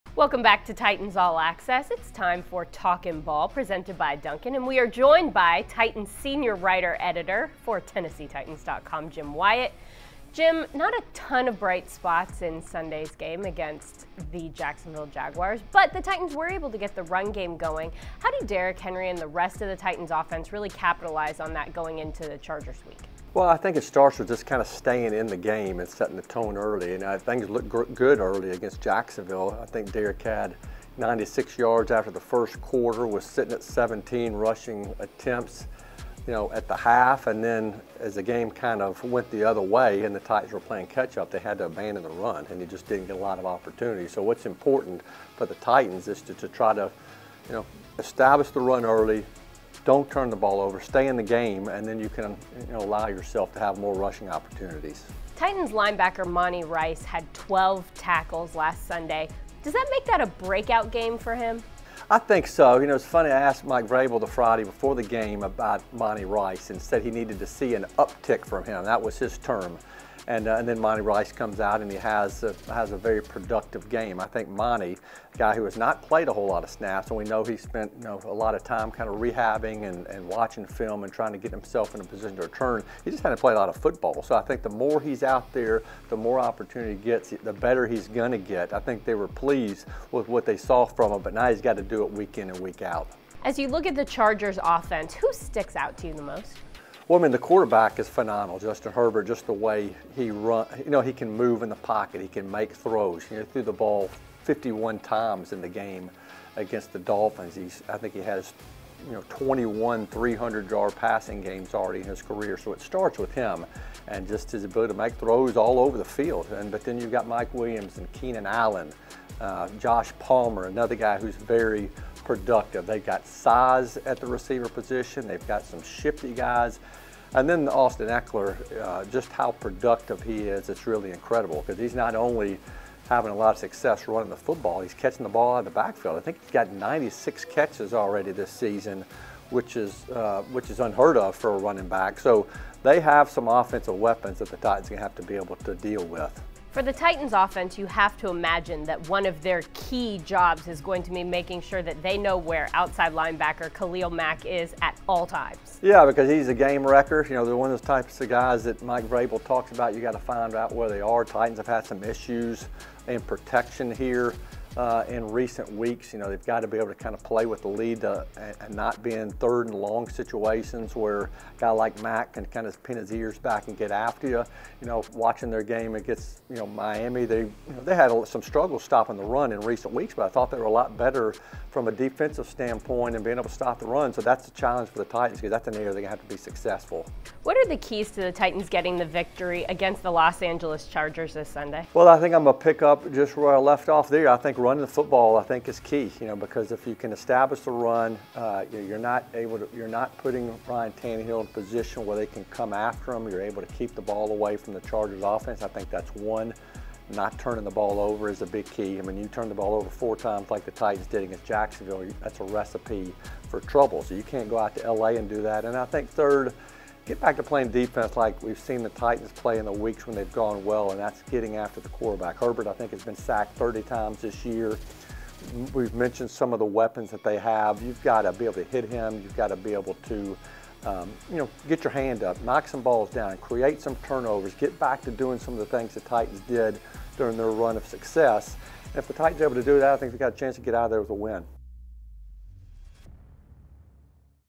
Keys for the Los Angeles Chargers | Talkin' Ball
Titans Senior Writer Jim Wyatt talks about the Titans Week 14 matchup against the Jacksonville Jaguars and looks ahead to the Week 15 matchup against the Los Angeles Chargers during Talkin' Ball presented by Dunkin'.